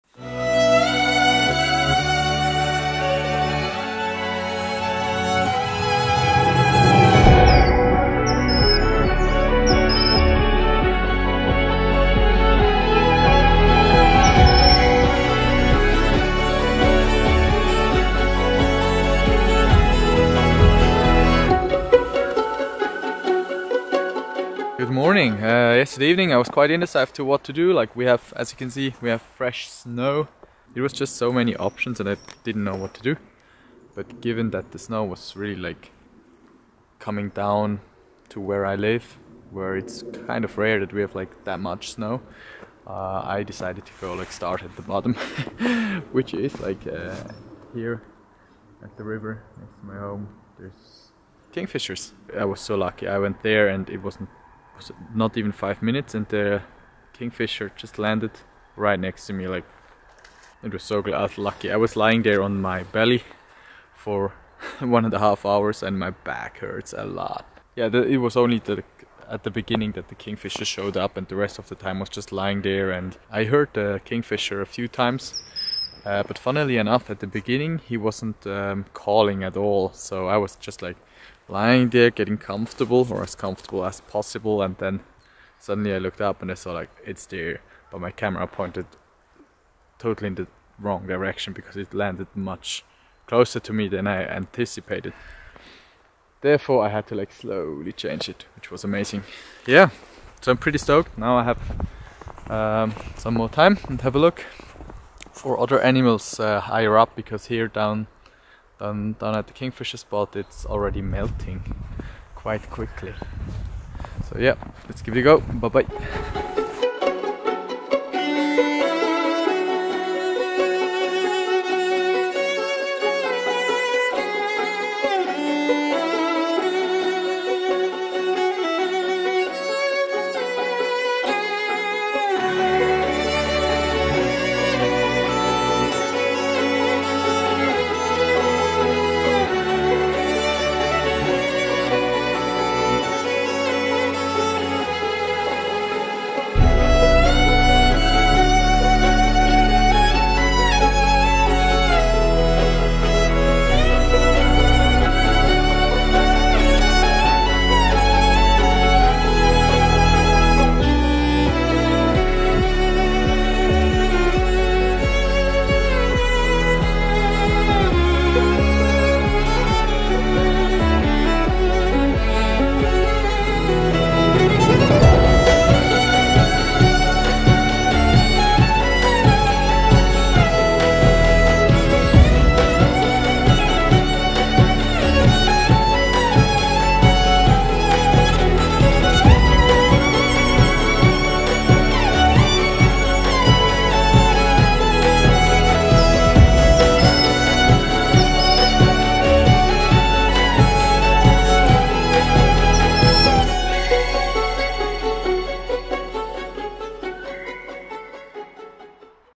Photographing Kingfisher in Winter 4K
Embark on a winter adventure along the frosty Aare River in Switzerland as we dive into the captivating world of kingfishers! Join us as we navigate the challenges of freezing temperatures, capturing the elusive beauty of these jewel-toned birds in their natural habitat. Brace yourself for breathtaking shots, tales of perseverance, and the magical allure of kingfishers thriving in the heart of winter. Don't miss out!